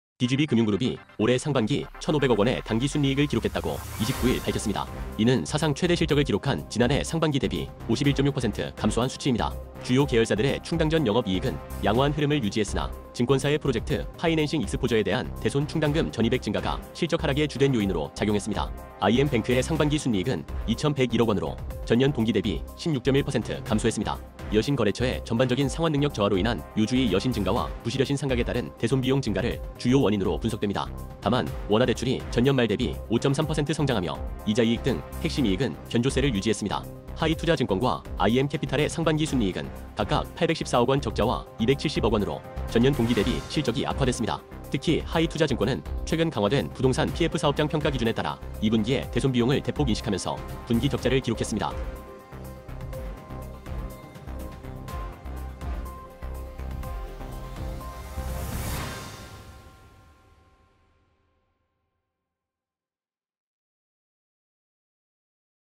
[알파경제TV] DGB금융, 상반기 순이익 1500억원…전년 대비 51.6% ↓
DGB금융그룹이 올해 상반기 1500억원의 당기순이익(지배주주 지분 기준)을 기록했다고 29일 밝혔습니다.

이는 사상 최대 실적을 기록한 지난해 상반기(3098억원) 대비 51.6% 감소한 수치입니다.

주요 계열사들의 충당전영업이익은 양호한 흐름을 유지했으나, 증권사의 프로젝트파이낸싱(PF) 익스포저(위험노출액)에 대한 대손충당금 전입액 증가가 실적 하락의 주된 요인으로 작용했습니다.

iM뱅크의 상반기 순이익은 2101억원으로, 전년 동기 대비 16.1% 감소했습니다.

여신거래처의 전반적인 상환능력 저하로 인한 요주의 여신 증가와 부실여신 상각에 따른 대손비용 증가를 주요 원인으로 분석됩니다.

다만 원화대출이 전년 말 대비 5.3% 성장하며 이자이익 등 핵심이익은 견조세를 유지했습니다.

하이투자증권과 iM캐피탈의 상반기 순이익은 각각 814억원 적자와 270억원으로, 전년 동기 대비 실적이 악화됐습니다.

특히 하이투자증권은 최근 강화된 부동산 PF 사업장 평가 기준에 따라 2분기에 대손비용을 대폭 인식하면서 분기 적자를 기록했습니다.